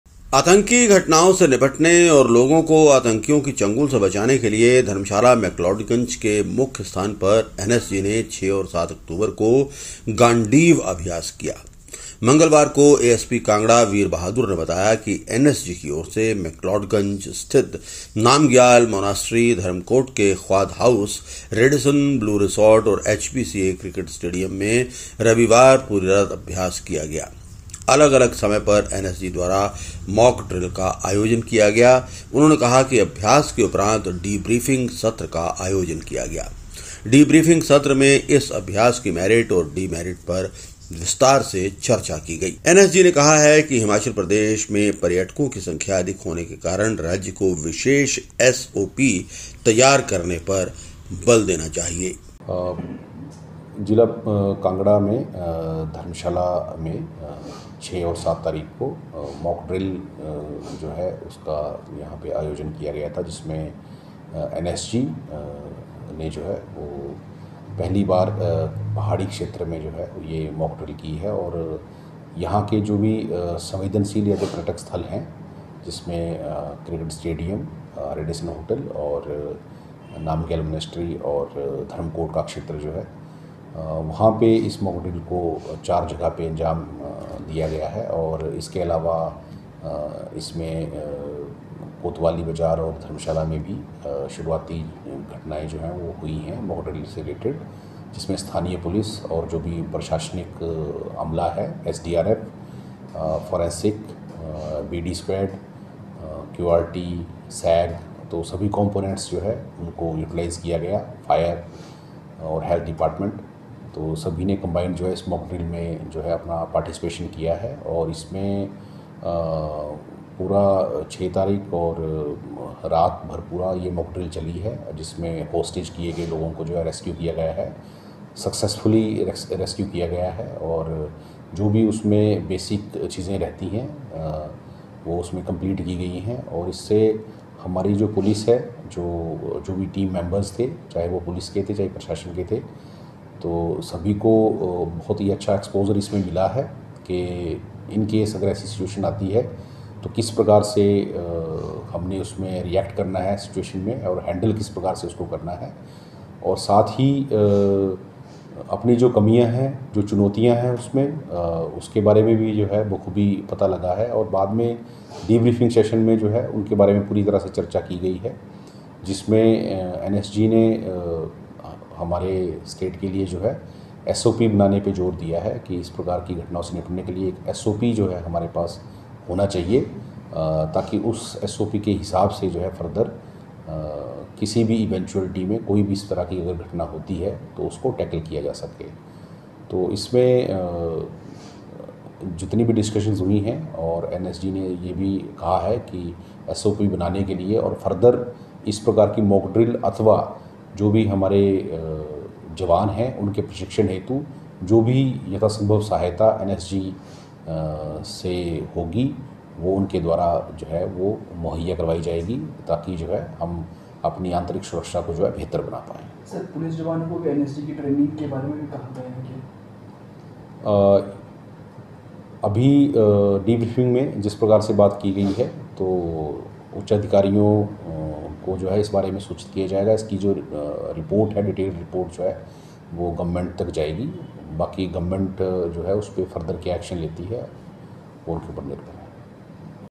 एनएसजी ने धर्मशाला में की मॉकड्रिल | NSG | Mock Drill
एनएसजी ने धर्मशाला में की मॉकड्रिल - Join us as we bring you exclusive coverage of the NSG's thrilling mock drill in the picturesque town of Dharamshala! 🏞️ Witness the precision and expertise of India's elite National Security Guard as they simulate real-life scenarios to ensure our safety. This in-depth report not only showcases the NSG's tactical prowess but also highlights the importance of preparedness in today's world. Don't miss out on this gripping story that blends action with insight. 🔔